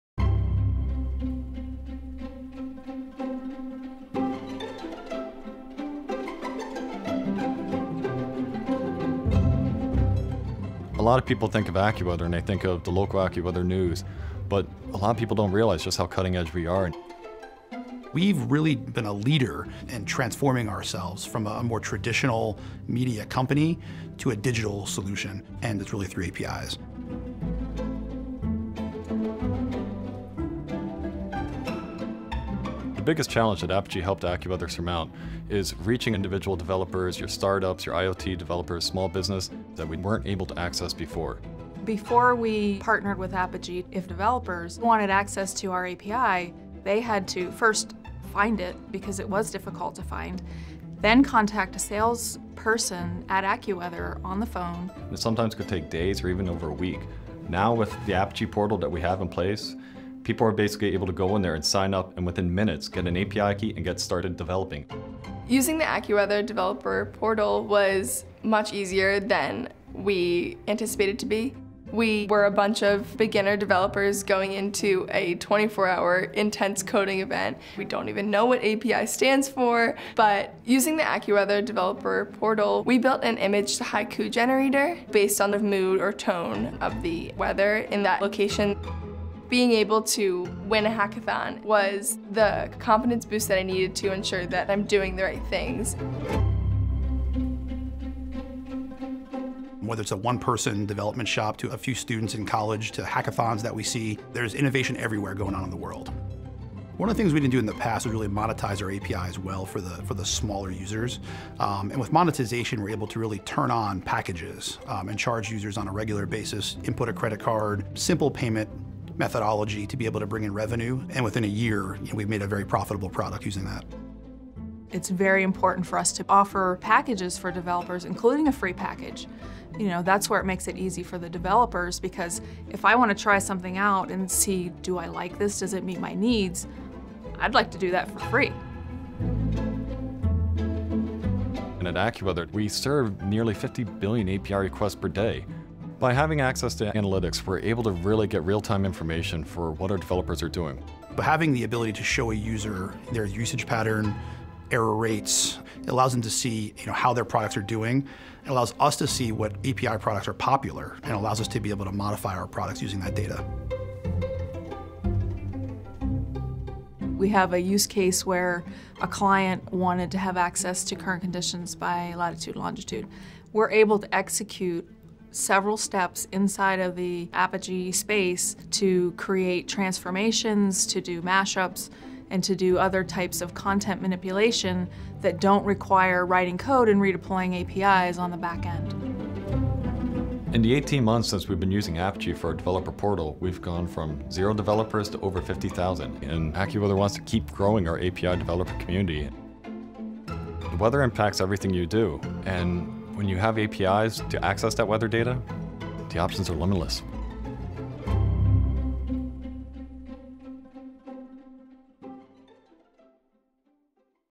AccuWeather: Delivering the Weather with API Management
Learn how AccuWeather, the leading source of weather forecasts and warnings, benefits from several features and aspects of the Apigee API platform, including: the developer portal, API analytics, API monetization, and orchestration.